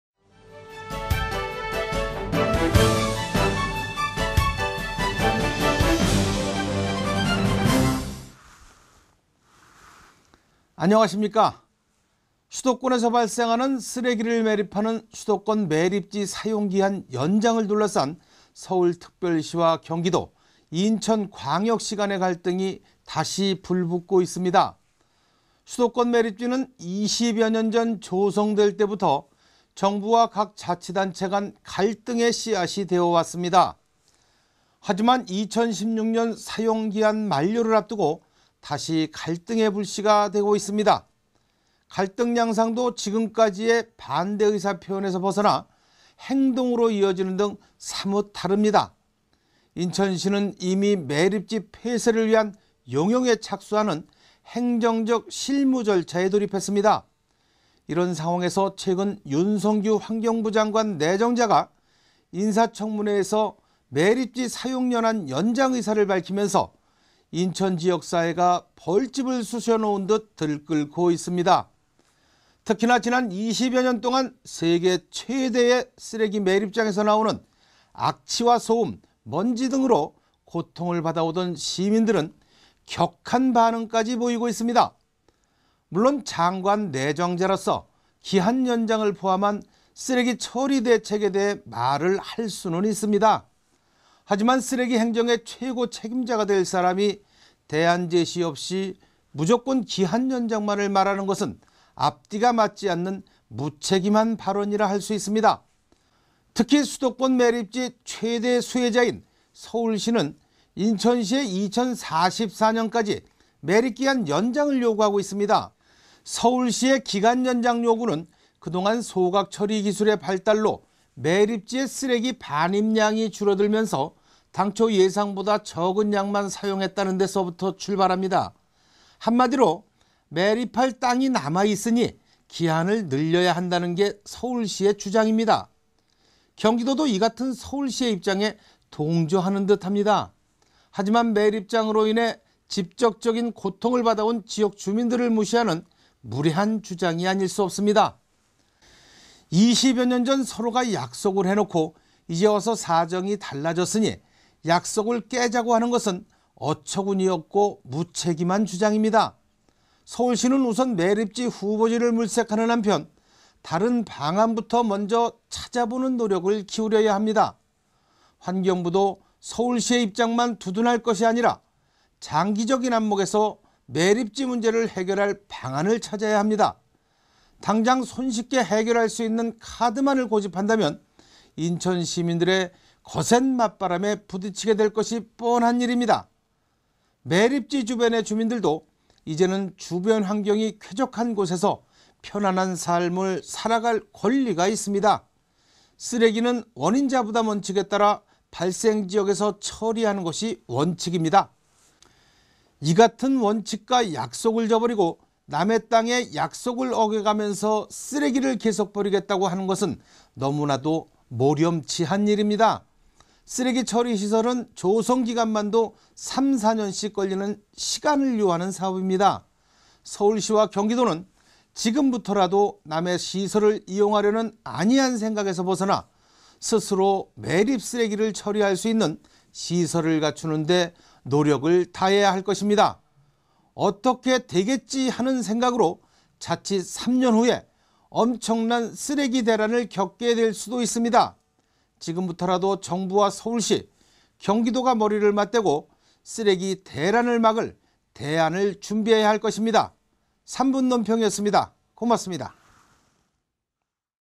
(경기방송 3분논평) 수도권매립지, 기간연장 안된다
수도권에서 발생하는 쓰레기를 매립하는 수도권 매립지사용기한 연장을 둘러싼 서울특별시와 경기도, 인천광역시간의 갈등이 다시 불붙고 있습니다.

수도권 매립지는 20여년 전 조성될 때부터 정부와 각 자치단체간 갈등의 씨앗이 되어왔습니다.

하지만 2016년 사용기한 만료를 앞두고 다시 갈등의 불씨가 되고 있습니다.

갈등양상도 지금까지의 반대의사 표현에서 벗어나 행동으로 이어지는 등 사뭇 다릅니다.

인천시는 이미 매립지 폐쇄를 위한 용역에 착수하는 행정적 실무절차에 돌입했습니다.

이런 상황에서 최근 윤성규 환경부장관 내정자가 인사청문회에서 매립지 사용연한 연장의사를 밝히면서 인천 지역사회가 벌집을 쑤셔놓은 듯 들끓고 있습니다.

특히나 지난 20여년동안 세계 최대의 쓰레기 매립장에서 나오는 악취와 소음, 먼지 등으로 고통을 받아오던 시민들은 격한 반응까지 보이고 있습니다.

물론 장관 내정자로서 기한 연장을 포함한 쓰레기 처리대책에 대해 말을 할수는 있습니다.

하지만 쓰레기 행정의 최고 책임자가 될 사람이 대안제시 없이 무조건 기한연장만을 말하는 것은 앞뒤가 맞지 않는 무책임한 발언이라 할수 있습니다.

특히 수도권 매립지 최대 수혜자인 서울시는 인천시에 2044년까지 매립기한 연장을 요구하고 있습니다.

서울시의 기한연장 요구는 그동안 소각처리 기술의 발달로 매립지에 쓰레기 반입량이 줄어들면서 당초 예상보다 적은 량만 사용했다는 데서부터 출발합니다..

한마디로 매립할 땅이 남아 있으니 기한을 늘려야 한다는 게 서울시의 주장입니다.

경기도도 이같은 서울시의 입장에 동조하는 듯 합니다.

하지만 매립장으로 인해 직접적인 고통을 받아 온 지역주민들을 무시하는 무례한 주장이 아닐 수 없습니다.

20여년 전 서로가 약속을 해 놓고 이제와서 사정이 달라졌으니 약속을 깨자고 하는 것은 어처구니 없고 무책임한 주장입니다.

서울시는 우선 매립지 후보지를 물색하는 한편 다른 방안부터 먼저 찾아보는 노력을 기울여야 합니다

환경부도 서울시의 입장만 두둔할 것이 아니라 장기적인 안목에서 매립지 문제를 해결할 방안을 찾아야 합니다.

당장 손쉽게 해결할 수 있는 카드만을 고집한다면 인천시민들의 거센 맞바람에 부딪히게 될 것이 뻔합니다.

매립지 주변의 주민들도 이제는 주변 환경이 쾌적한 곳에서 편안한 삶을 살아갈 권리가 있습니다.

쓰레기는 원인자 부담원칙에 따라 발생지역에서 처리하는 것이 원칙입니다.

이같은 원칙과 약속을 저버리고 남의 땅에 약속을 어겨가면서 쓰레기를 계속 버리겠다고 하는 것은 너무나도 몰염치한 일입니다.

쓰레기 처리시설은 조성기간만도 3~4년씩 걸리는 시간을 요하는 사업입니다.

서울시와 경기도는 지금부터라도 남의 시설을 이용하려는 안이한 생각에서 벗어나 스스로 매립쓰레기를 처리할 수 있는 시설을 갖추는데 노력을 다해야 할 것입니다.

어떻게 되겠지하는 생각으로 자칫 3년후에 엄청난 쓰레기 대란을 겪게 될수도 있습니다.

지금부터라도 정부와 서울시, 경기도가 머리를 맞대고 쓰레기 대란을 막을 대안을 준비해야 할 것입니다.